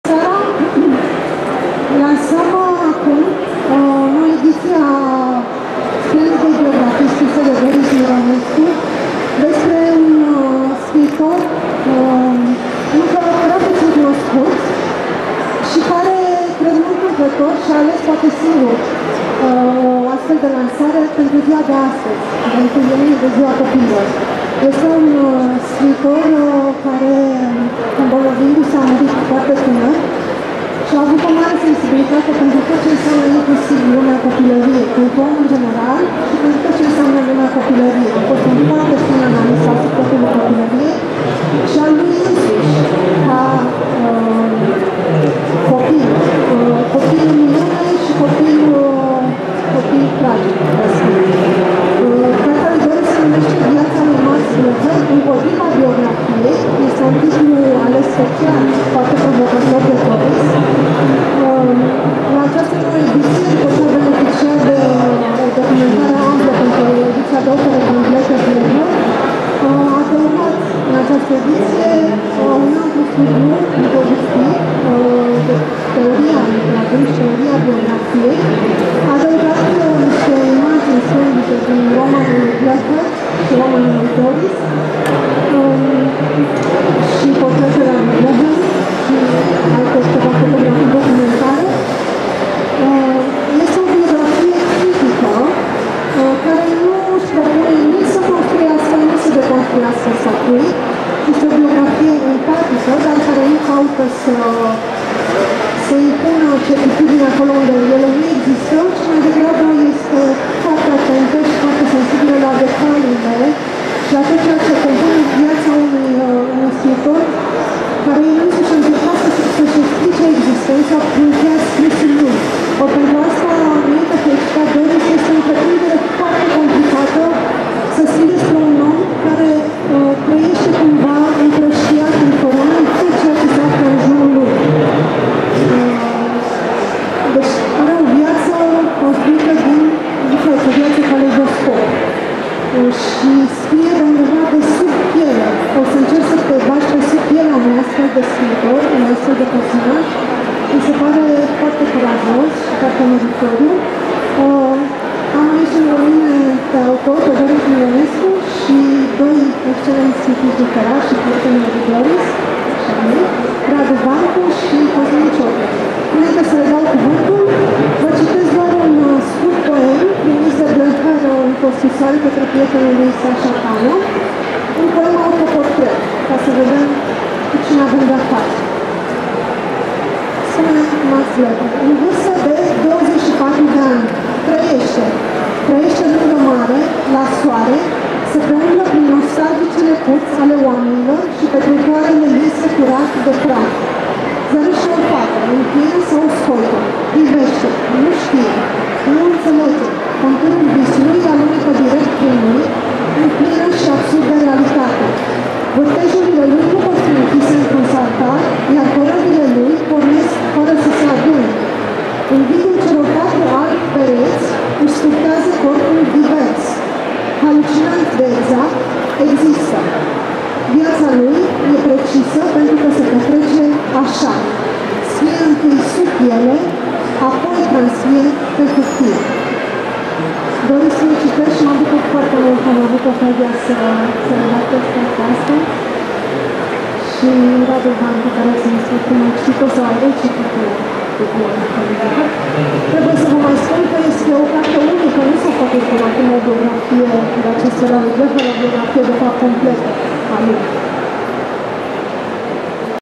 (1) Marieva Ionescu - Lansare "Viața lui M. Blecher. Împotriva biografiei" de Doris Mironescu
În data de 1 iunie 2018 a avut loc la standul editurii Humanitas din cadrul Bookfest (Romexpo, București, 2018) lansarea cărții "Viața lui M. Blecher. Împotriva biografiei", de Doris Mironescu.

Vorbitori: Doris Mironescu, Radu Vancu, Cosmin Ciotloș, Marieva Ionescu.

Mai jos, fotografii de la eveniment și înregistrarea video și audio a evenimentului.